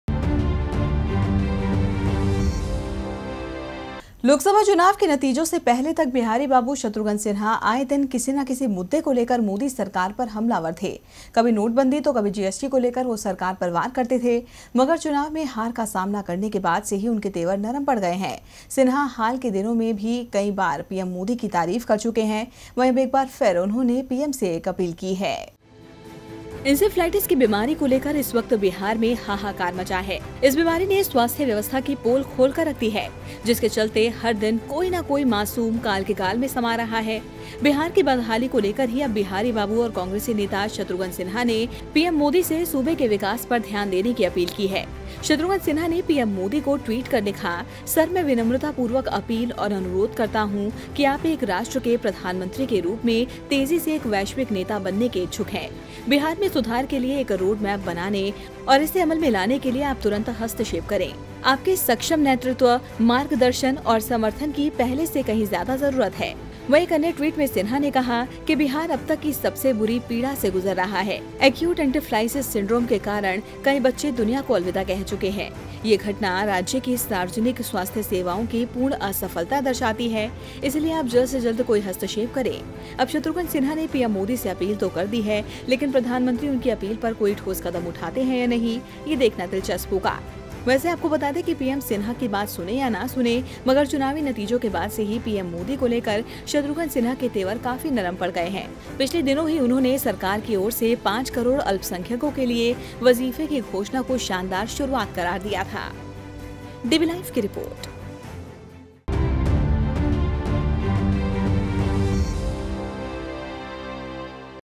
PM Modi पर नरम पड़े Shatrughan Sinha | Shatrughan Sinha ने की PM Modi से अपील
लोकसभा चुनाव के नतीजों से पहले तक बिहारी बाबू शत्रुघ्न सिन्हा आए दिन किसी ना किसी मुद्दे को लेकर मोदी सरकार पर हमलावर थे...कभी नोटबंदी तो कभी जीएसटी को लेकर वो सरकार पर वार करते थे...मगर  चुनाव में हार का सामना करने के बाद से ही उनके तेवर नरम पड़ गए हैं... सिन्हा हाल के दिनों में कई बार पीएम मोदी की तारीफ कर चुके हैं. वहीं अब एक बार फिर उन्होंने पीएम से एक अपील की है।